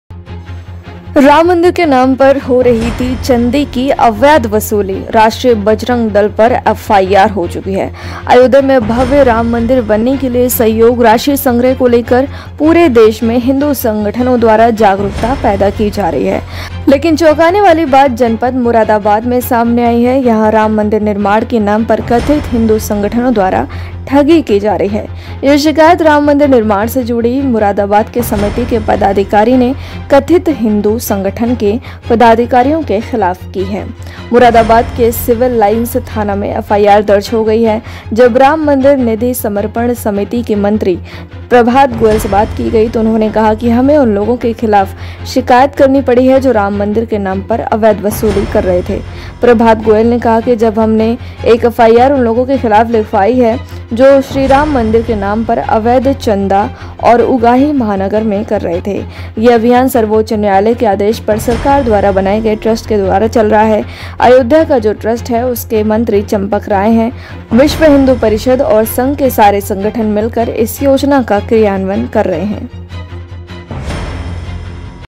राम मंदिर के नाम पर अयोध्या में चंदे की अवैध वसूली, राष्ट्रीय बजरंग दल की शर्मनाक हरकत
Awareness is being created by Hindu organizations across the country regarding the collection of funds for the construction of a grand Ram temple in Ayodhya, but the shocking thing…